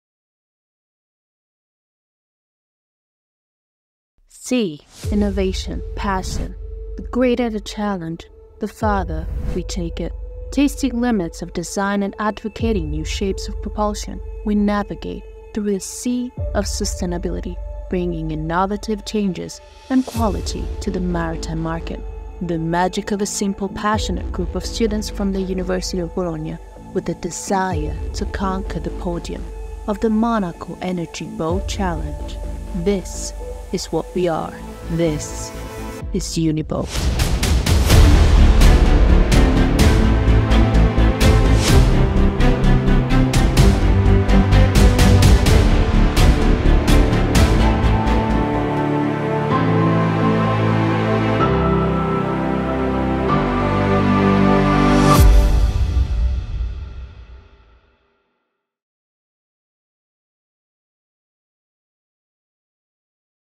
Uniboat - Alma Mater Studiorum Università di Bologna - Italy
🇫🇷 | UniBoAT est un projet innovant d'enseignement et de recherche né au sein du Département de Génie Industriel de l'Université Alma Mater Studiorum de Bologne en octobre 2019. L'équipe est dédiée au développement et à la mise en œuvre d'un bateau entièrement électrique et respectueux de l'environnement, grâce à des solutions durables. Nous participons pour la quatrième fois afin de symboliser et de démontrer nos efforts dans le développement et l'amélioration de ce domaine de recherche.

🇬🇧 | UniBoAT is an innovative teaching and research project born inside the Industrial Engineering Department of the Alma Mater Studiorum University of Bologna in October 2019. The team is devoted to the development and implementation of an eco-friendly and fully electric boat, through sustainable solutions. We are participating for the fourth time as an emblem and demonstration of our efforts into developing and improving this field of research.